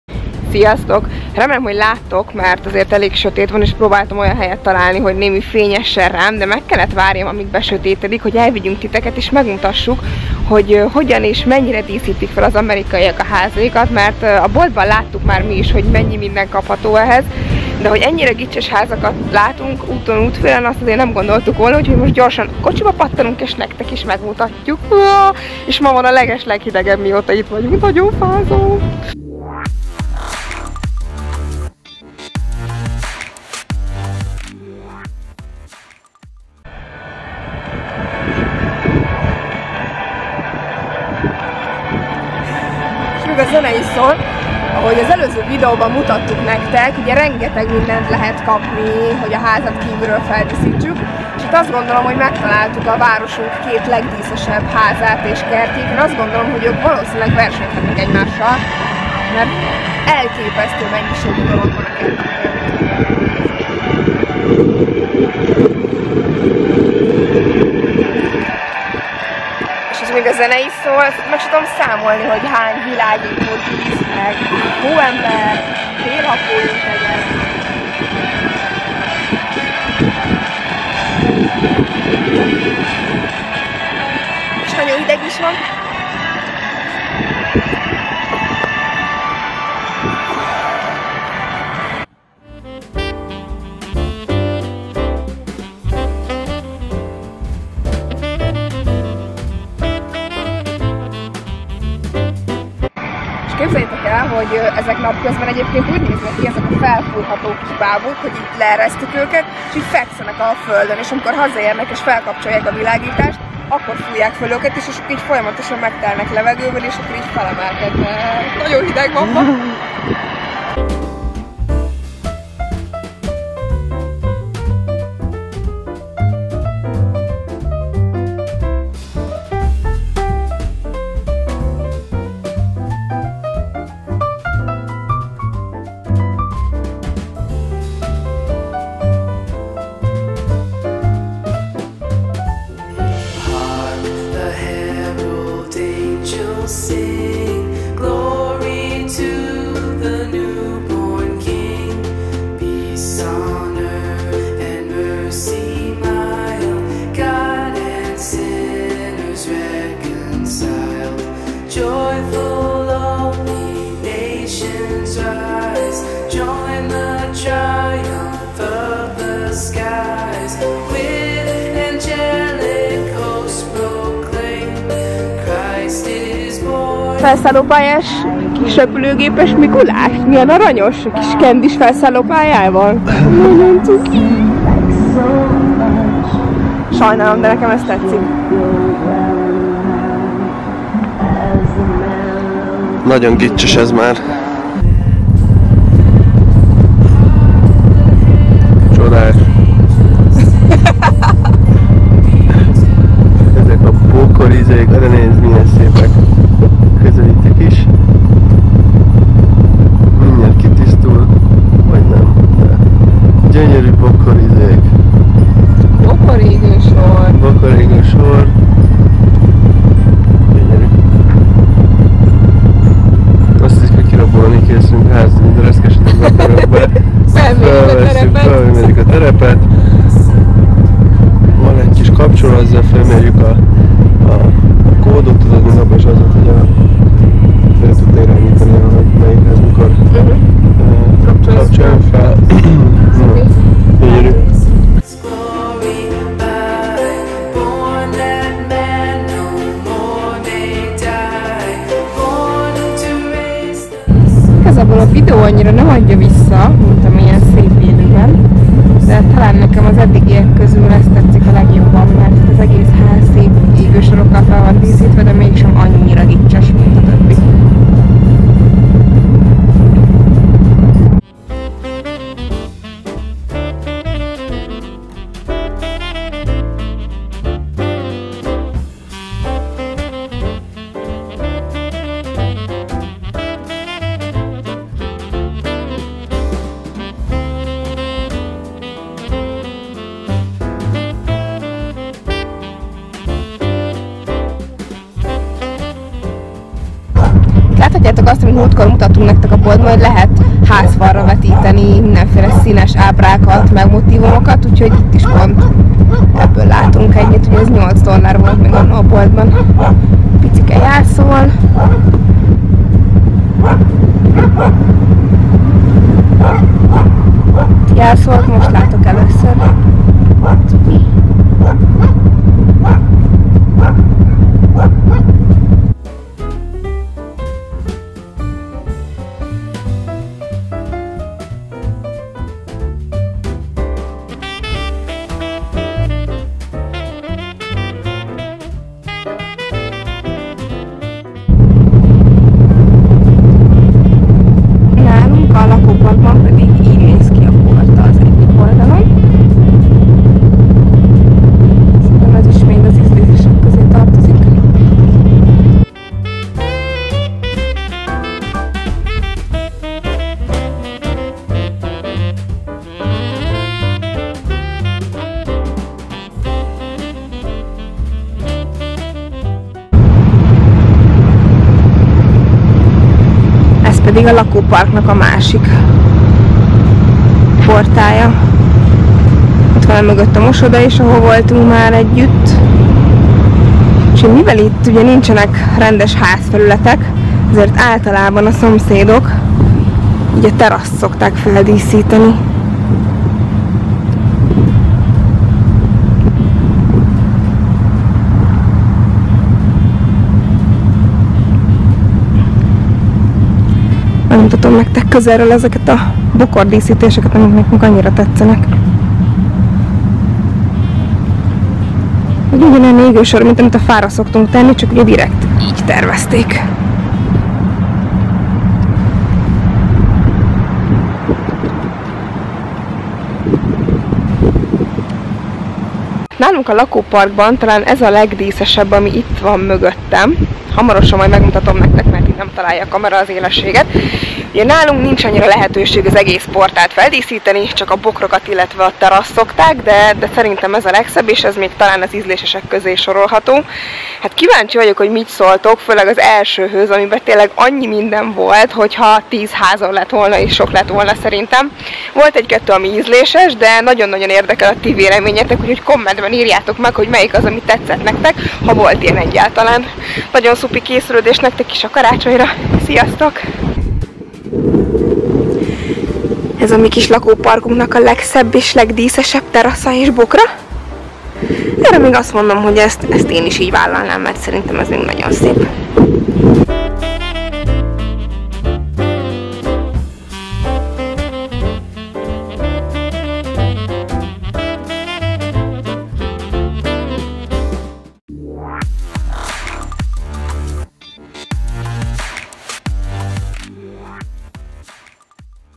Life in the USA- Házdíszítés Karácsonyra amerikai módra/ Elképesztő csillogás
Mindig is kíváncsi voltam, hogy hogyan néznek ki előben a Karácsonyra feldíszített amerikai házak. Megmondom őszintén, hogy én bírom a túlzásokat, de néhány háznál nekem is tátva maradt a szám. Az érdekel a legjobban, hogy Ti mit szóltok hozzá! Írjátok meg kommentben!